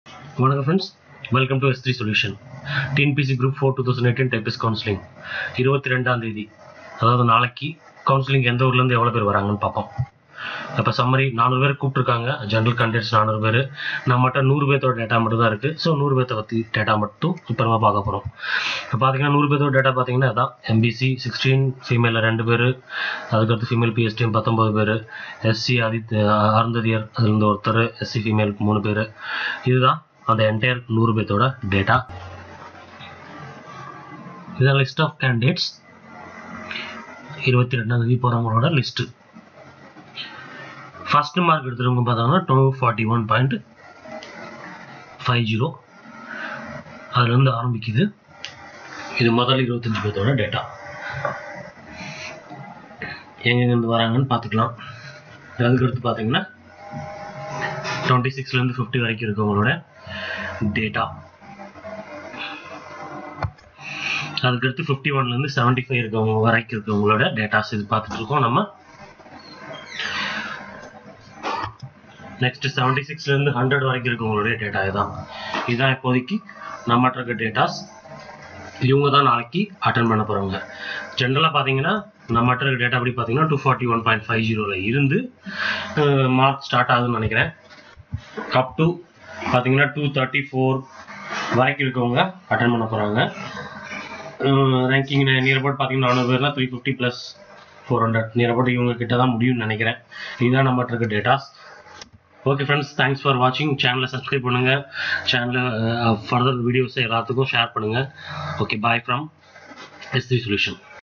TNPSC GROUP –IV 2018 COUNSELINGக்கு 22-01-2019 தேதி எந்த ஊரில் இருந்து எவ்வளவு பேர் வருவாங்க ?
TNPSC GROUP –IV   2018 TYPIST COUNSELING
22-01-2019 தேதி  எந்த ஊரில் இருந்து  எவ்வளவு பேர் வருவாங்க ?